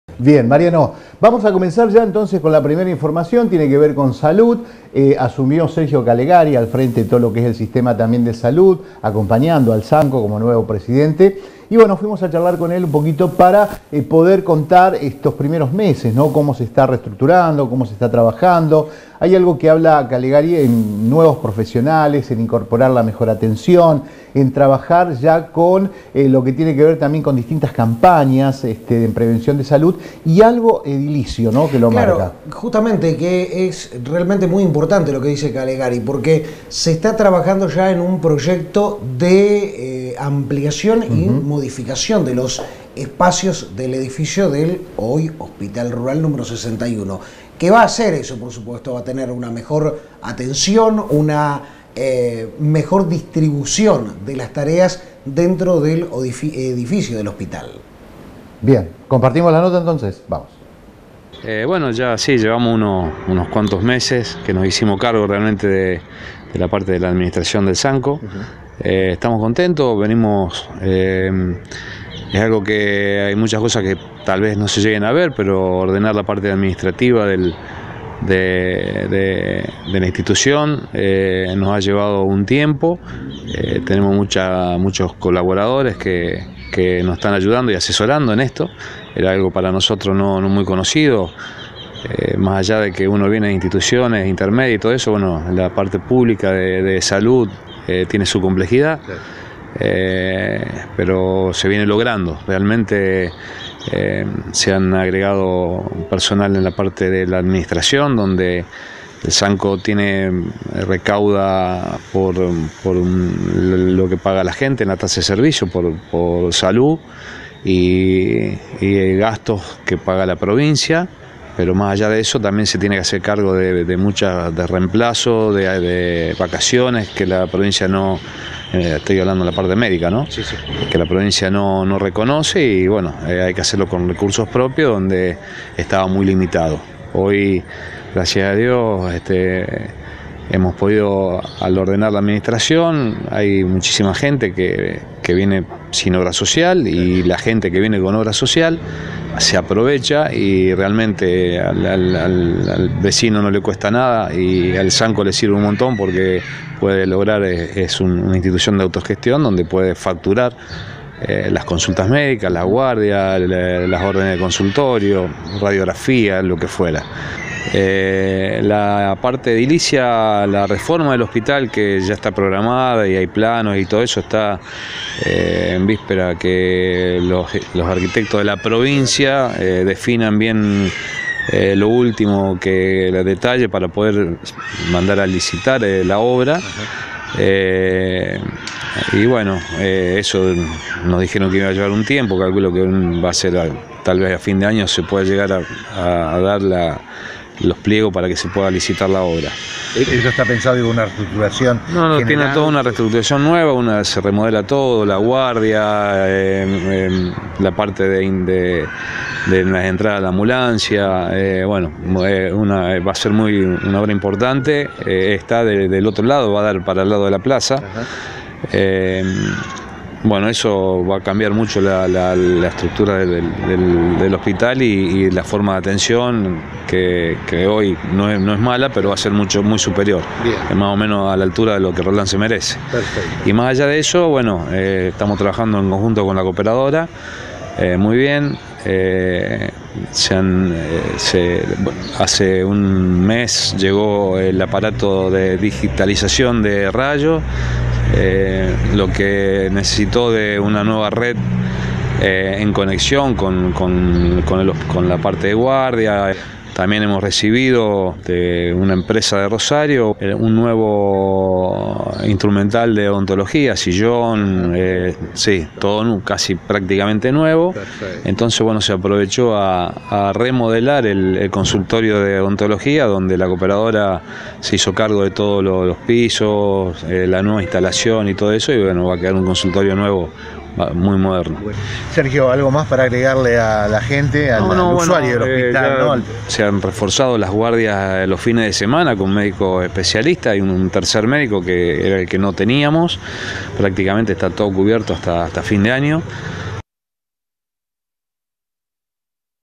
Actualidad y proyectos del SAMCO roldanense
El SAMCO roldanense aguarda las aprobaciones pertinentes para afrontar una serie de remodelaciones edilicias.